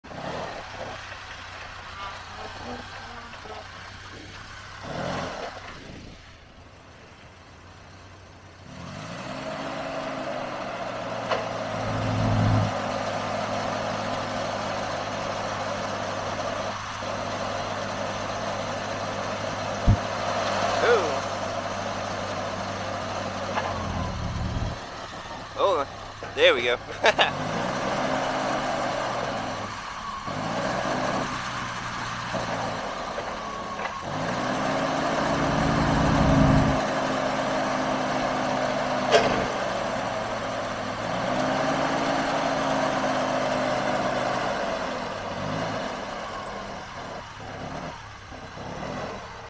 2073 CLARK C500-Y45 FORKLIFT
CLARK C500-Y45 FORKLIFT
4500LB CAPACITY, 3 STAGE MAST, LPG GAS ENGINE WITH BOTTLE, SN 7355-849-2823